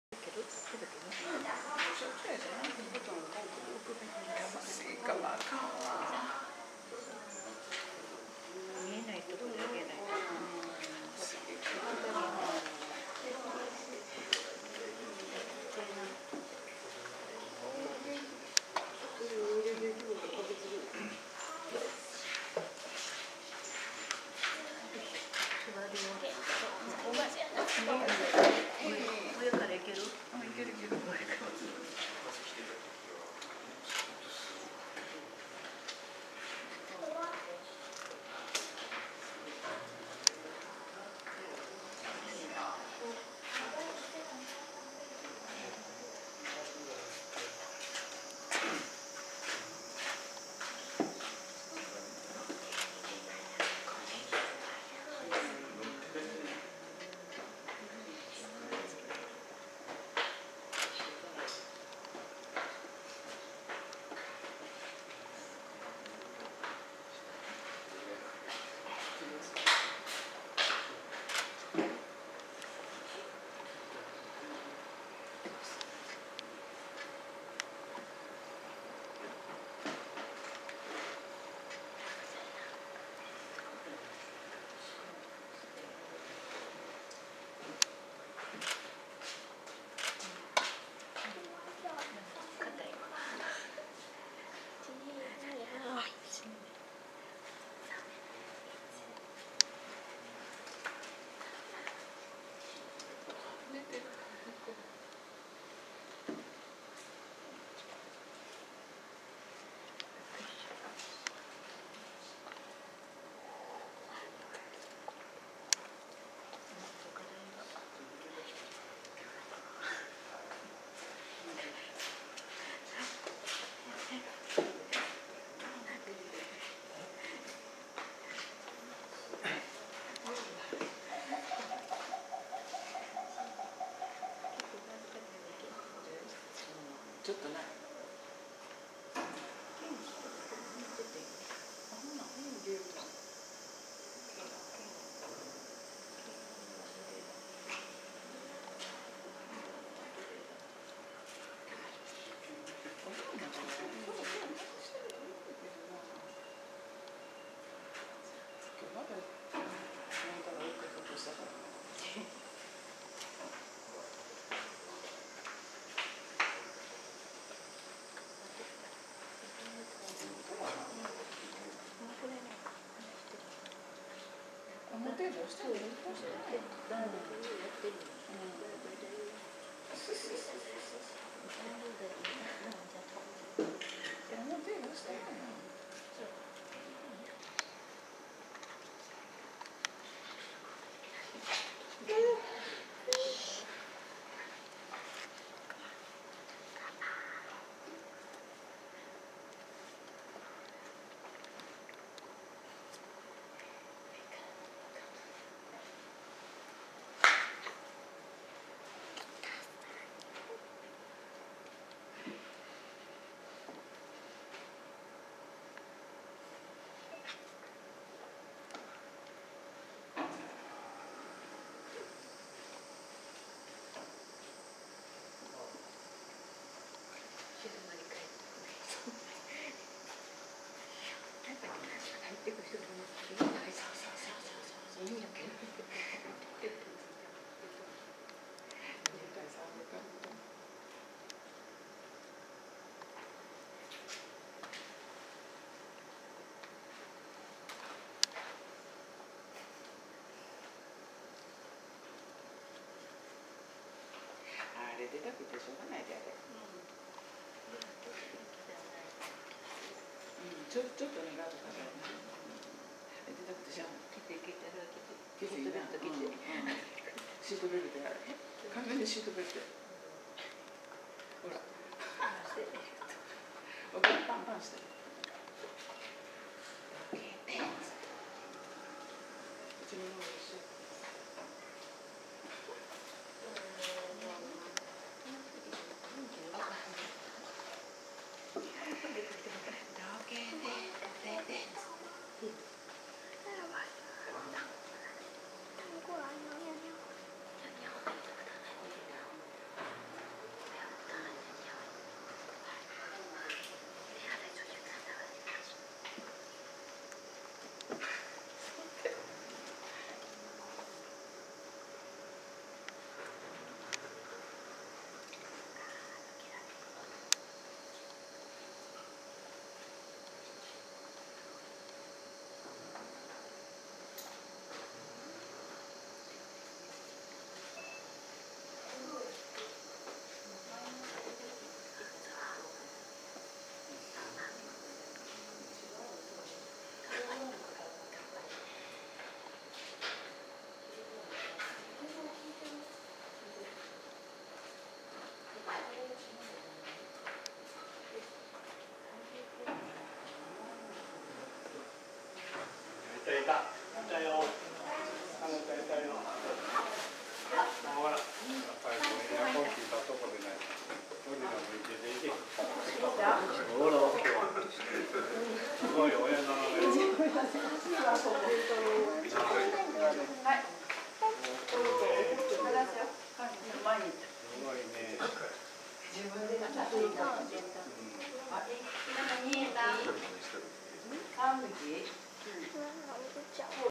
【京都市動物園】キンタロウ「もう放して」 Kintaro: "Mama, let go of it"
腕の中に抱え込み放さないゲンキママにキンタロウが、「ママ、もう放して」とばかり、ママのうでをたたいたりしています。
2019/8/11撮影
Kintaro is hitting the mom ’s navel, “Mom, let go” to Genki mom who does n’t hold it in her arms.2019/8/11